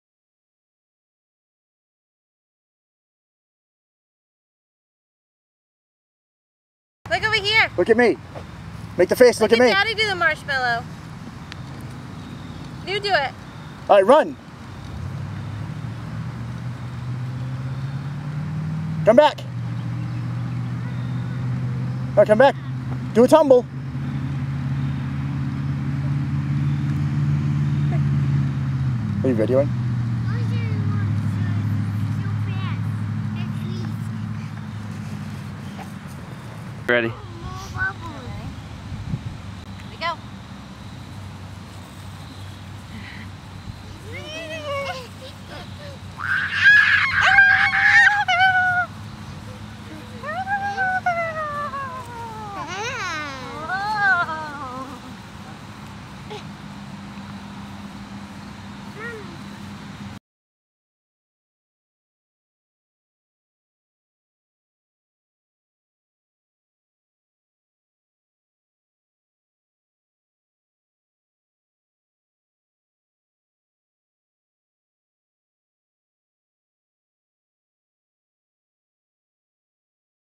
How to do the Marshmallow dance and fly like superman tutorial
This is the official Marshmallow dance tutorial.  As well as the how to fly like super man tutorial of awesomeness!